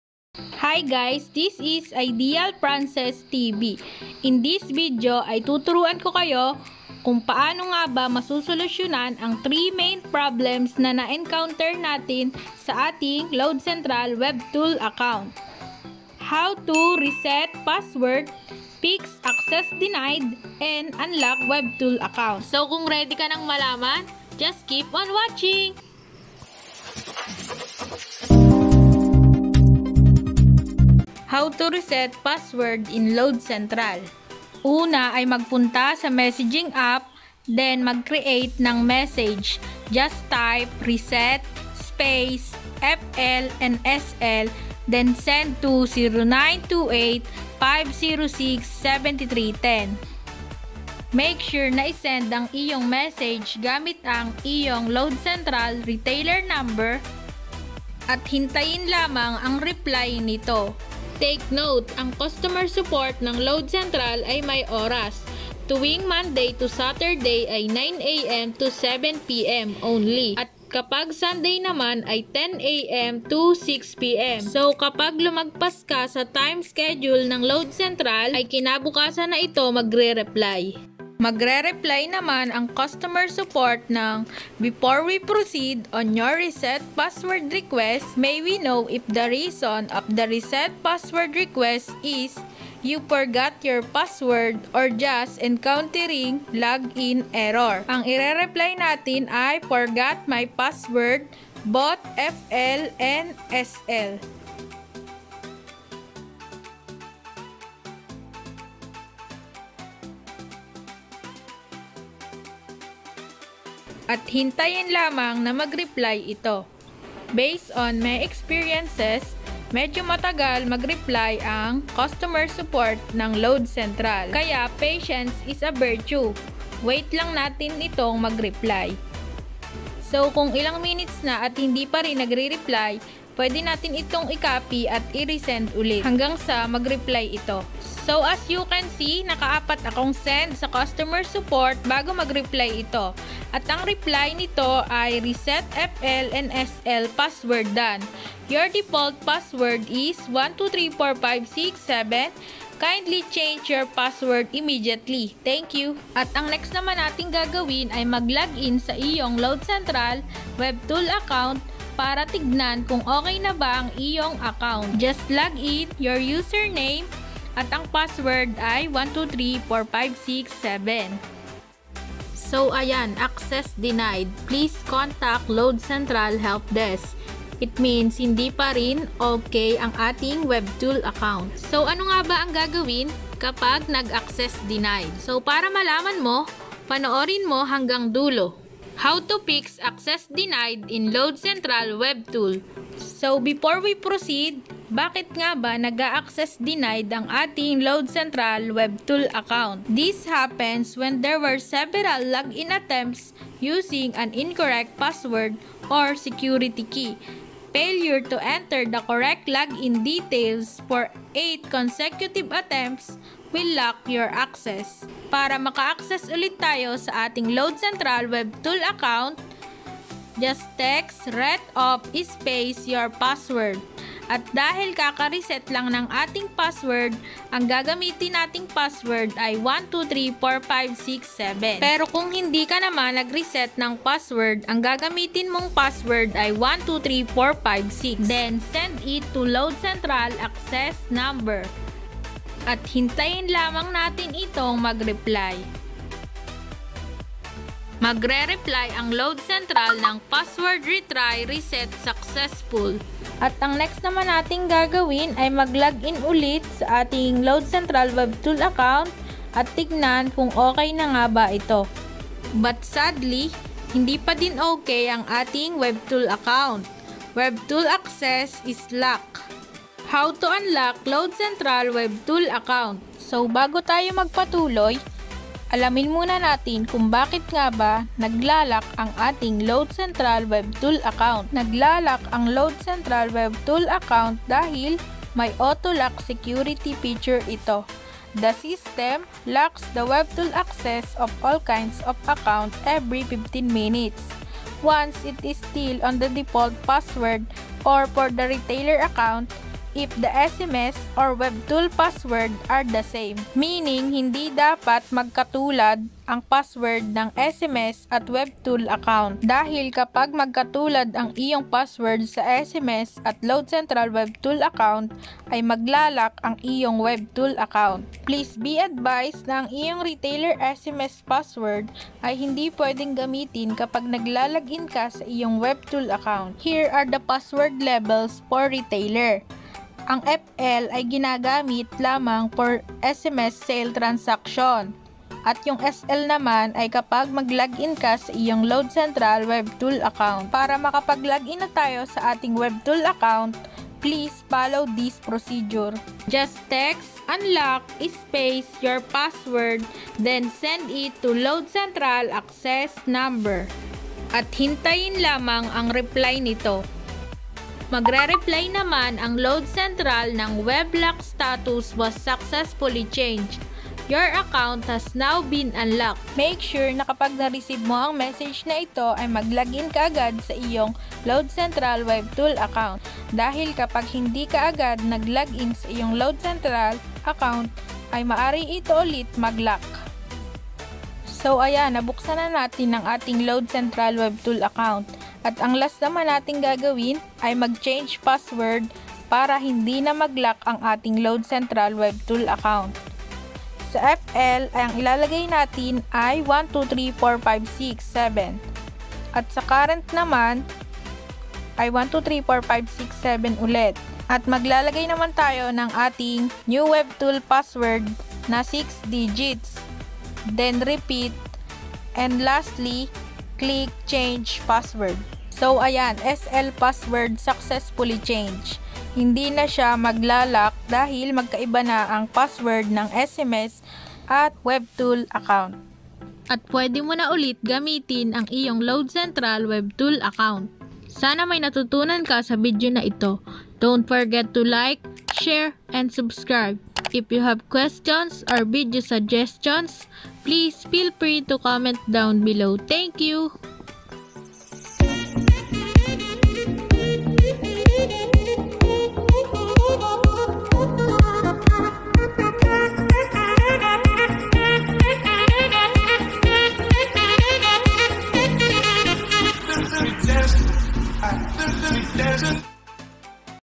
HOW TO RESET PASSWORD, FIX ACCESS DENIED AND UNLOCK LOAD CENTRAL WEBTOOL?
In this video, tuturuan ko kayo kung paano mag reset ng password, mag ayos ng access denied at mag unlock ng loadcentral webtool.

Time codes:

0:00 Intro
0:31 How to Reset Password?
3:08 How to Fix Access Denied?
4:29 How to Unlock Load Central Webtool?
7:44 end